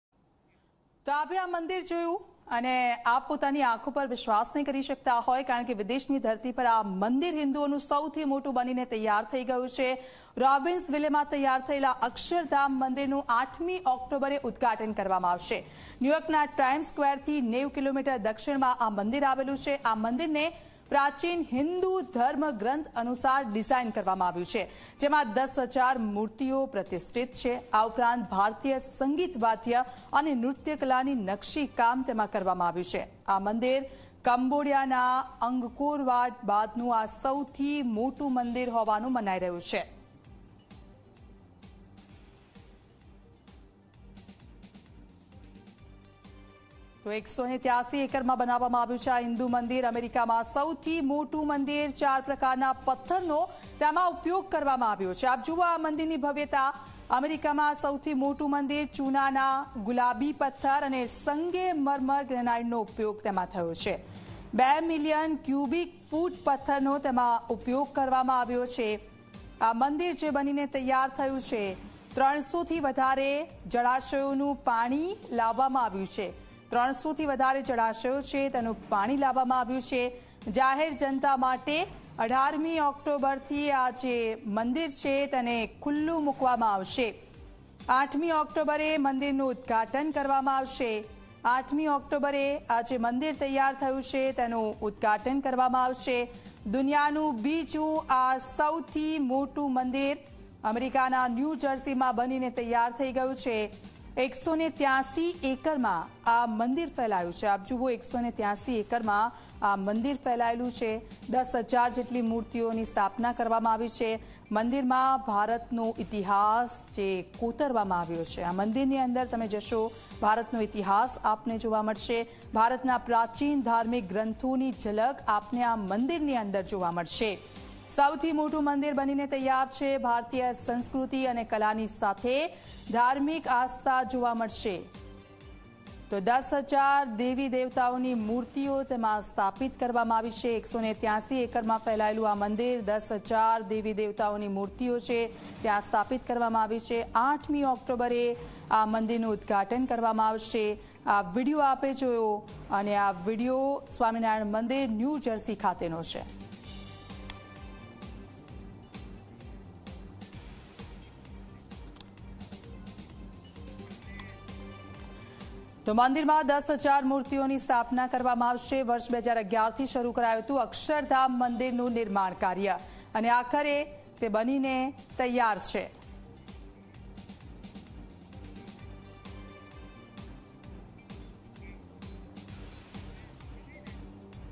World's 2nd-largest Hindu temple outside India to be inaugurated in US on Oct 8
અમેરિકાના ન્યૂ જર્સીમાં હિંદુઓનું સૌથી મોટું મંદિર બનીને તૈયાર થઈ ગયું છે. રોબિન્સ વિલેમાં તૈયાર થયેલા અક્ષરધામ મંદિરનું 8 ઓક્ટોબરે ઉદ્ધાટન કરવામાં આવશે. ન્યૂયોર્કના ટાઈમ્સ સ્ક્વેરથી 90 કિમી દક્ષિણમાં આ મંદિર આવેલું છે. આ મંદિરને પ્રાચીન હિન્દુ ધર્મગ્રંથ અનુસાર ડિઝાઇન કરવામાં આવ્યું છે. જેમાં 10 હજાર મૂર્તિઓ પ્રતિષ્ઠિત છે... આ ઉપરાંત ભારતીય સંગીત વાદ્ય અને નૃત્યકલાની નકશીકામ કરવામાં આવ્યું છે... આ મંદિર કમ્બોડિયાના અંગકોરવાટ બાદનું સૌથી મોટું મંદિર હોવાનું માનવામાં આવી રહ્યું છે. અક્ષરધામ હિન્દુ મંદિરને વાસ્તુકલા ધ્યાનમાં રાખીને નિર્માણ કરવામાં આવ્યું છે. આ અદ્વિતીય માંનીરની ડિઝાઇનમાં એક મુખ્ય મંદિર, 12 ઉપ મંદિર, 9 શિખર અને 9 પિરામીડ શિખરનો સમાવેશ થાય છે... અક્ષરધામ પારંપરિક પત્થર વાસ્તુકલાનો આ અત્યાર સુધીનો સૌથી મોટો અંડાકાર ગુંબજ છે.

અમેરિકામાં હિંદુઓનું સૌથી મોટું મંદિર બનીને તૈયાર છે... અને હવે તેના ઉદ્ધાટનની ઘડીઓ ગણાઈ રહી છે. હાલમાં મંદિર પ્રશાસન તરફથી તૈયાર થયેલા મંદિરનો એક વીડિયો રિલીઝ કરવામાં આવ્યો છે... જેને જોઈને તમે મંત્રમુગ્ધ થઈ જશો.